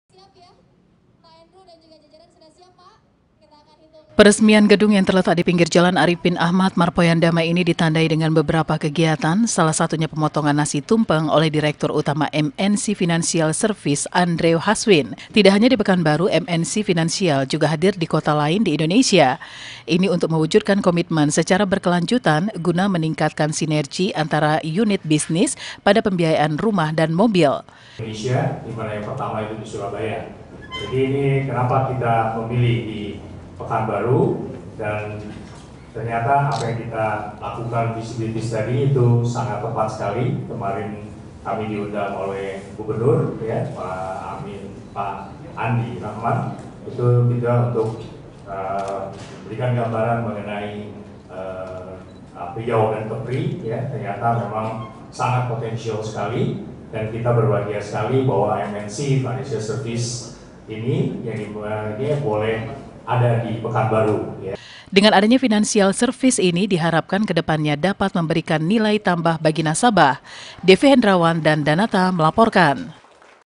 FINANCIAL SERVICES HADIR DI KOTA PEKANBARU
MNC Financial Services bergerak dalam bidang keuangan yang lengkap dan terintegrasi, hadir di Pekanbaru. Ini merupakan bentuk pelayanan dan menjawab kebutuhan masyarakat akan solusi keuangan.